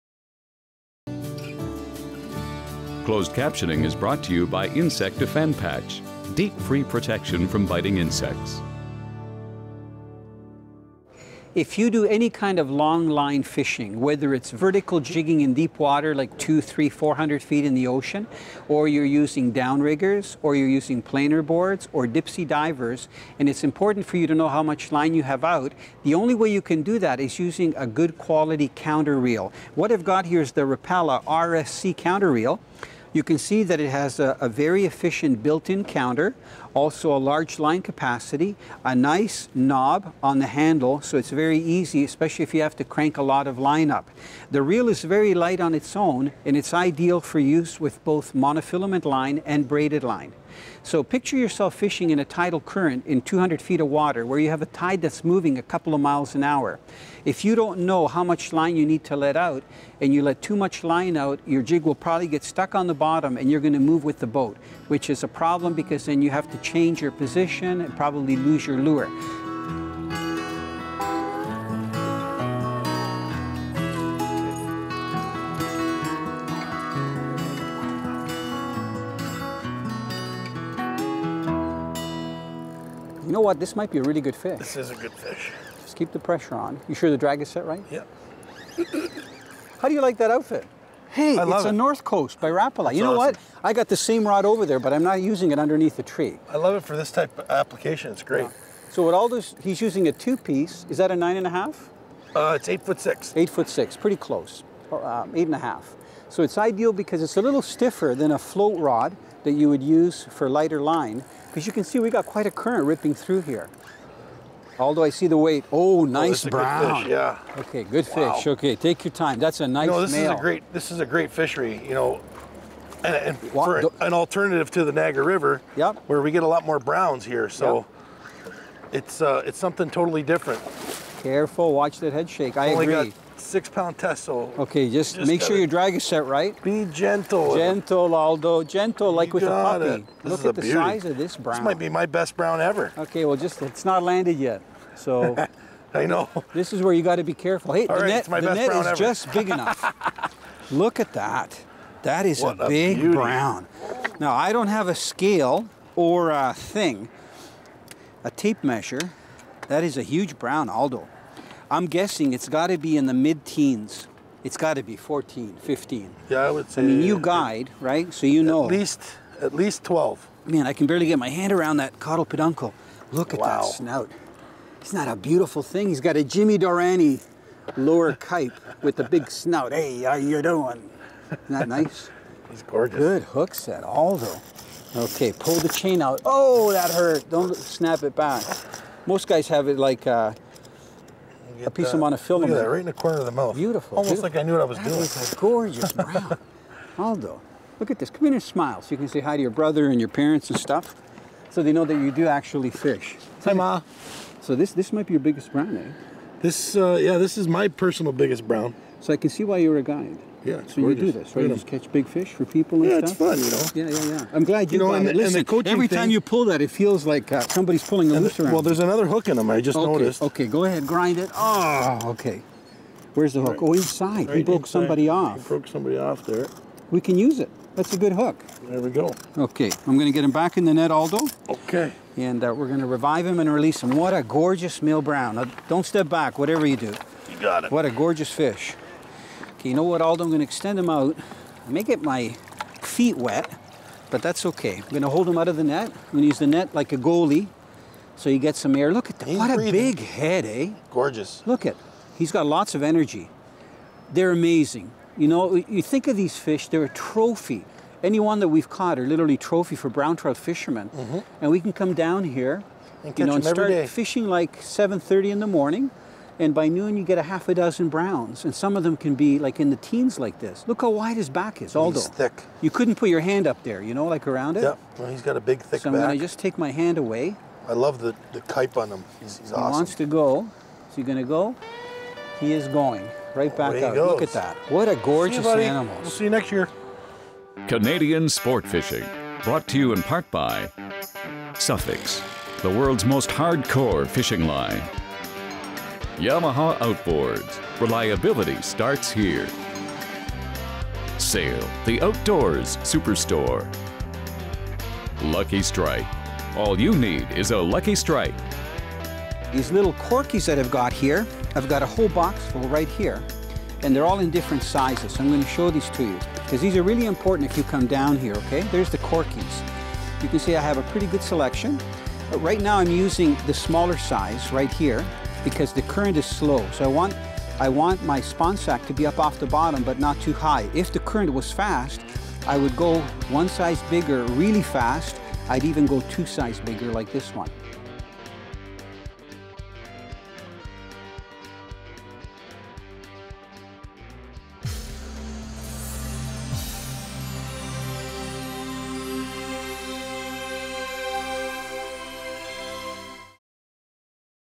Port Dalhousie Brown Trout, St. Catharines Ontario Part 4 of 4
Join Italo, and Aldo Nava and Roger Cannon as they fish for Trophy Brown Trout.